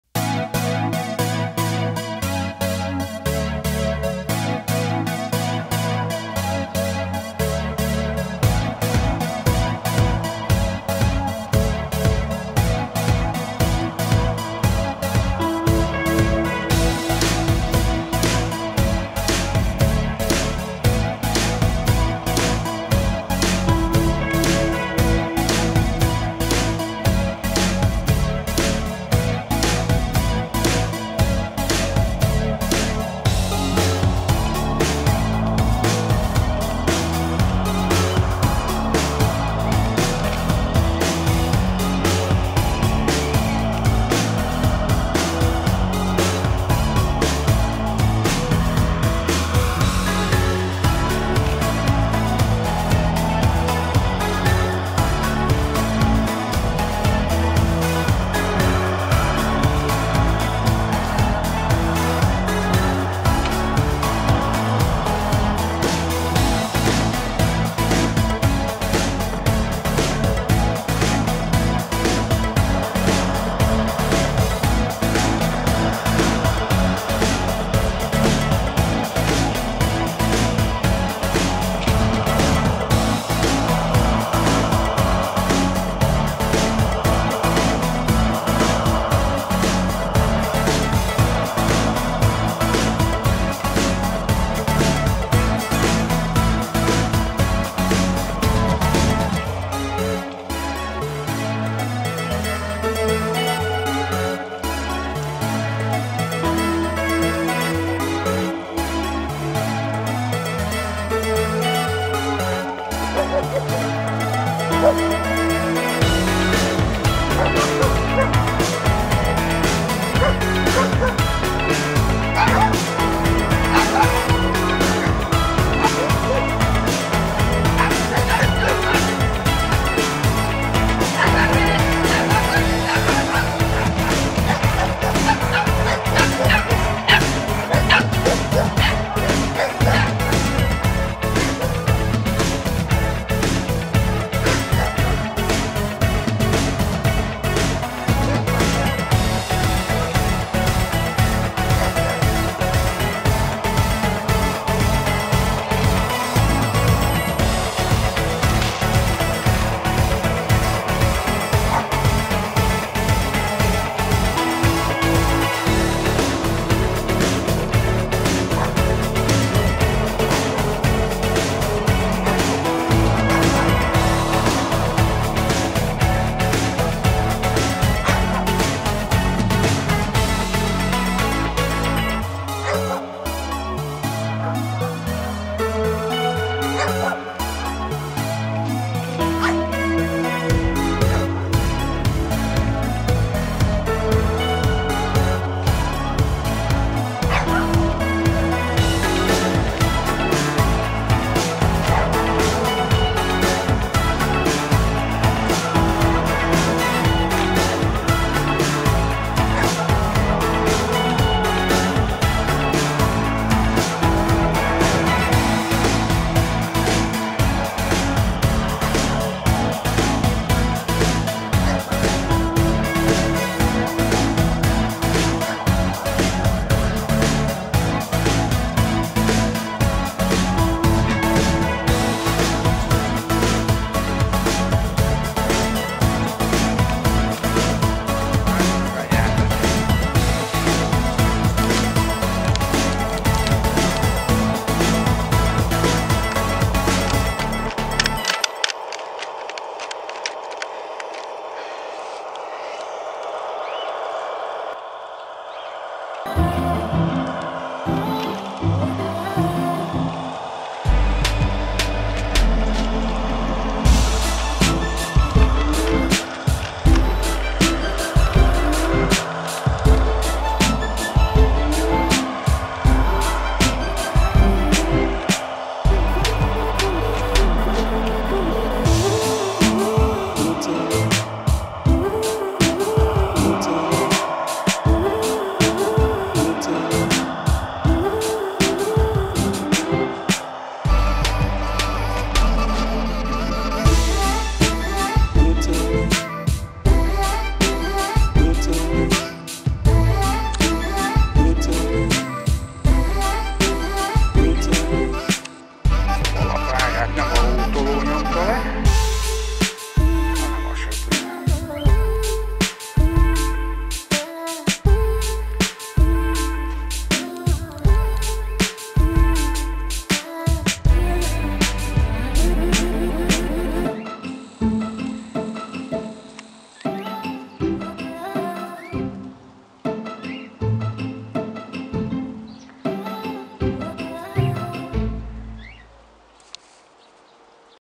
R.K.V.t.
Az én kerti vasutam. Esett valamennyi hó. Egy próbát meg érhet. De az eke is könnyű, azért meg meg pörög a nagy gép alatt is a kerék. Na meg a hó alatt a sok szemét ami a síneken van. Ennyi volt nem is foglalkozom tovább a hótolási filmekkel. Majd össze szedi a havat aki szétszórta. !